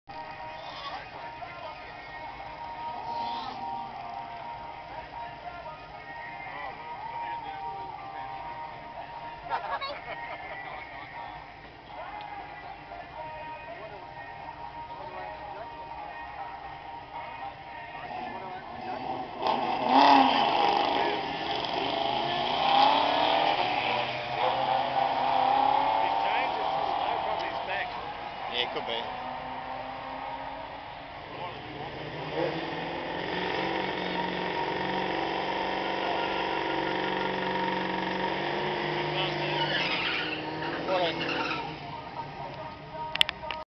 GT 351 and Valiant 225 slant 6
another close run 14.7@96 mph  to 17.1@79 mph